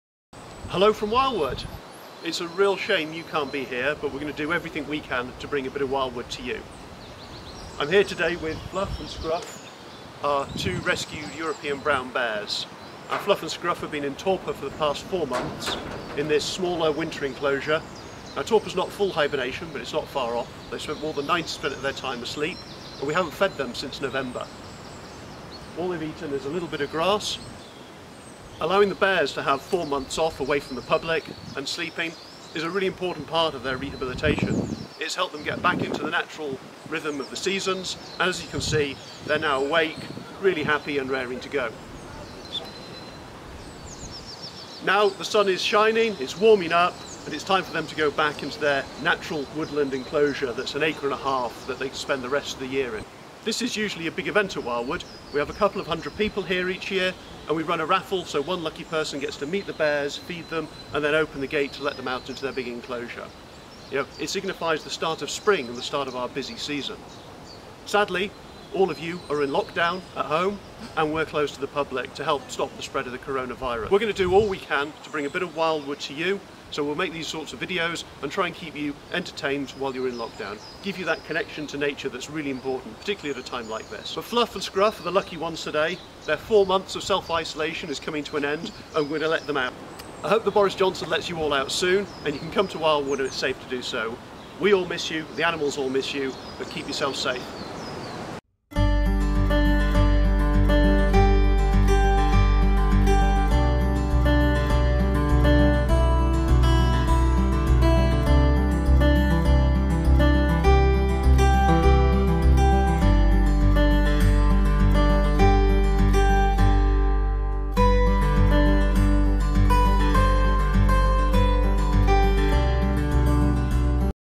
Wildwood bears back from winter torpor
It's springtime and after a long winter of torpor (half hibernation) Fluff and Scruff, the two rescue bears at Wildwood Trust in Kent, get access to their big woodland home for another season of fun and exploration.